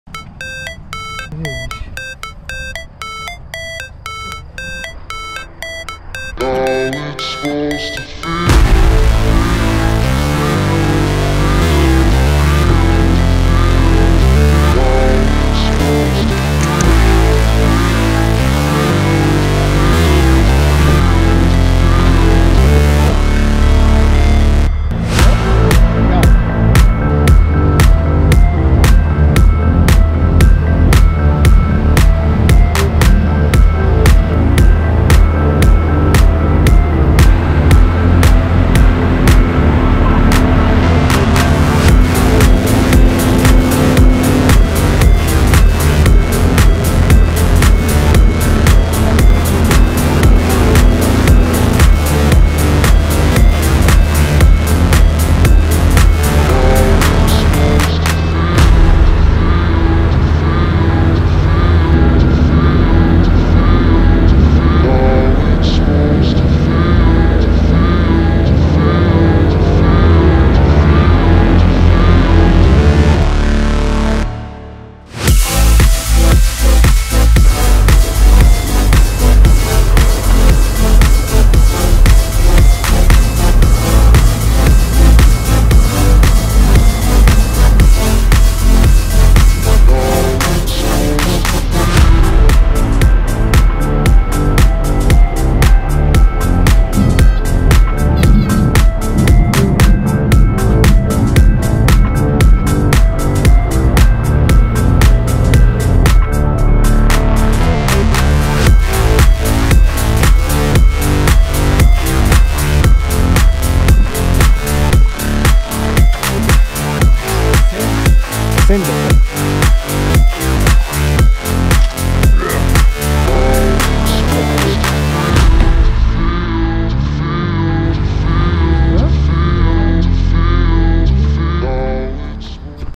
FUN FLIGHT With Small Plane Piper Tomahawk in Slovenija
With a pilot Nejc Jamšček we borrowed a plane in ajdovščina in slovenia and made an afternoon flight over the surrounding places.